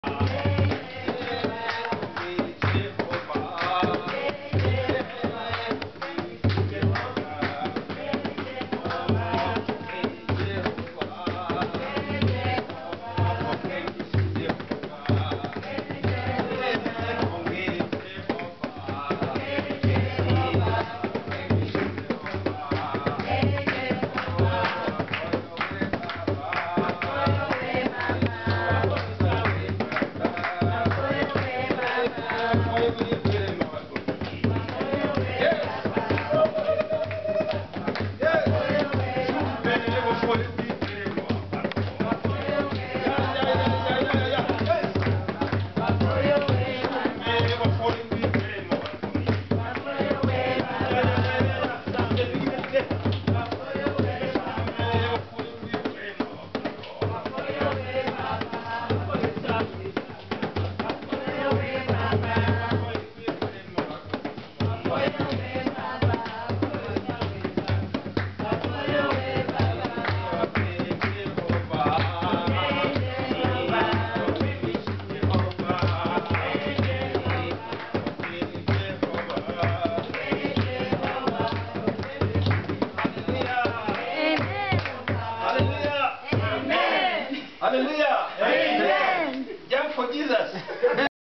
The SHIM Family Sings (and Dances) Thanksgiving
Papa and Mama O led the SHIM family in a special time of praise and worship during our monthly prayer day on Wednesday, Nov. 27, to say "thank you" to God, in about four different languages. :)